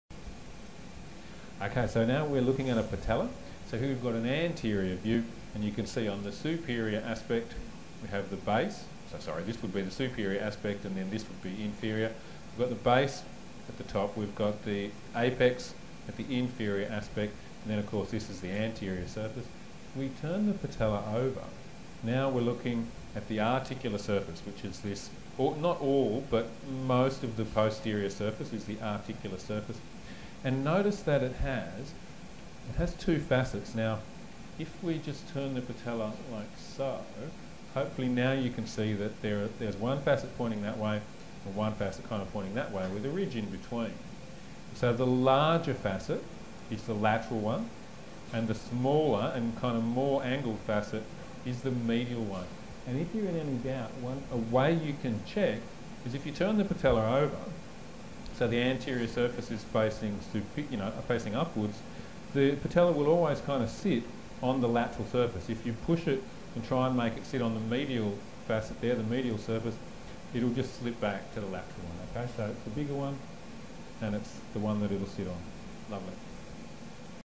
Patella
The anatomy of the patella for allied health students.